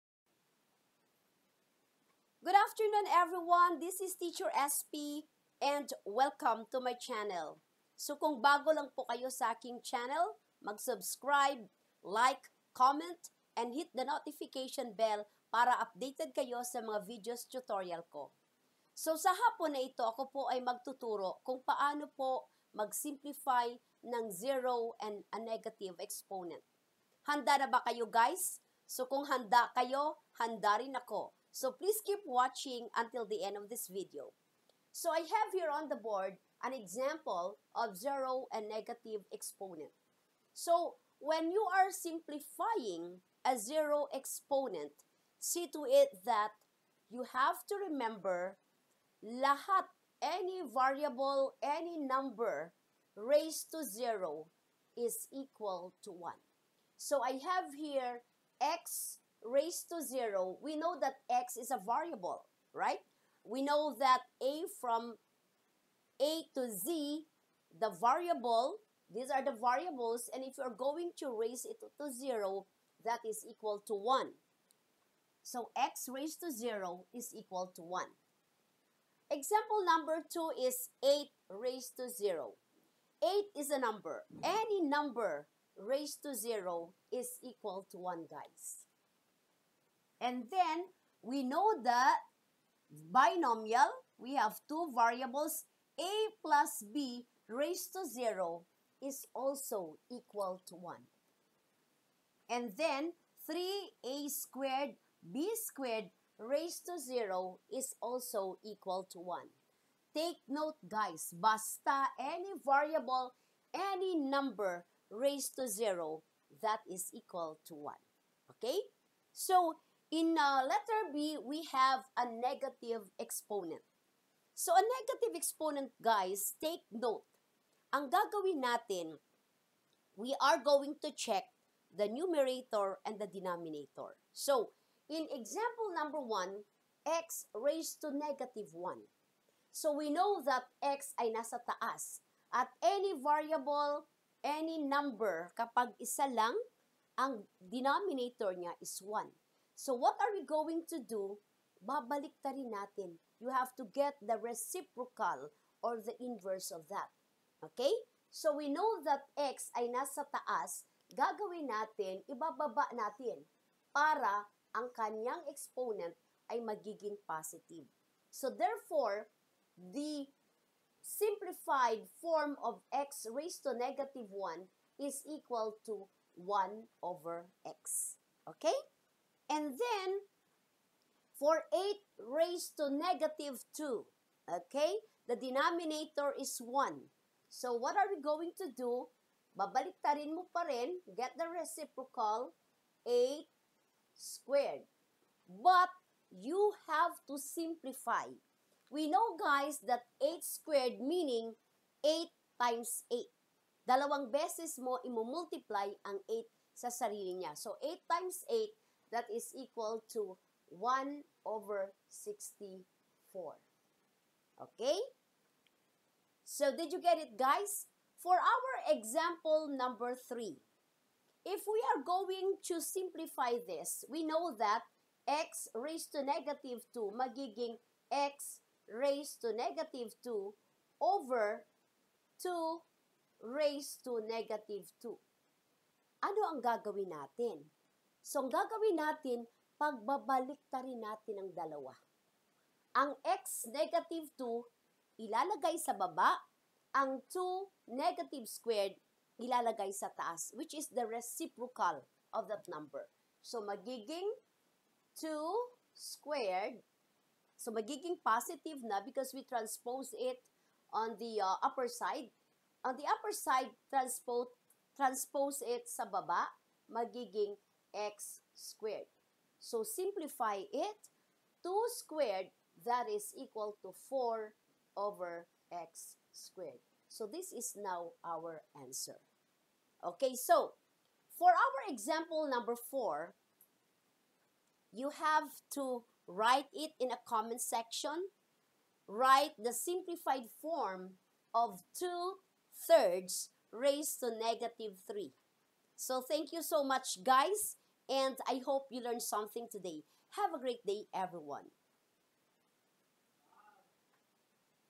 Zero and Negative Exponent- Simplifying
This video shows on how to learn to simplify a Zero and Negative exponent in an easy way.